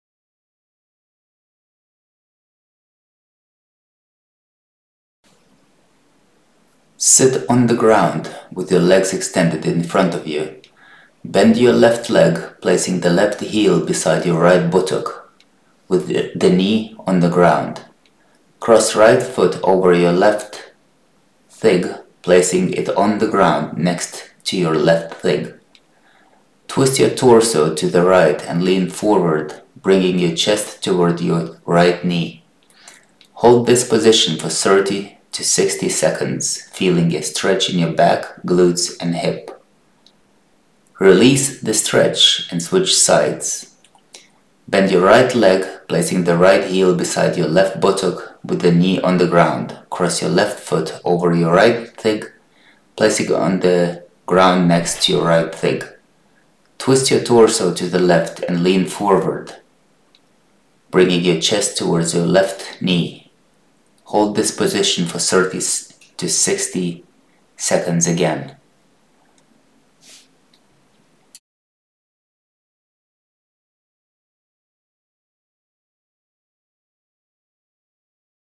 Deep Glute, Hip & Back Stretch | Seated Twist for Flexibility and Mobility #61
Unlock flexibility in your hips, glutes, and back with this seated twist stretch. This effective exercise helps relieve tension while promoting better mobility in your lower body and spine.

Sit on the ground with your legs extended in front of you.

Bend your left leg, placing the left heel beside your right buttock with your knee on the ground.
Cross your right foot over your left thigh, placing it on the ground next to your left thigh.
Twist your torso to the right and lean forward, bringing your chest toward your right knee.
Hold this stretch for 30-60 seconds, focusing on the stretch in your back, glutes, and hip.

Release the stretch and switch sides.
Bend your right leg, placing the right heel beside your left buttock with your knee on the ground.
Cross your left foot over your right thigh, placing it on the ground next to your right thigh.
Twist your torso to the left and lean forward, bringing your chest toward your left knee.
Hold this stretch for another 30-60 seconds.

This stretch releases tension in the back, glutes, and hips while improving flexibility and mobility in the lower body.
It’s an excellent stretch for athletes, desk workers, or anyone looking to improve flexibility and relieve tightness.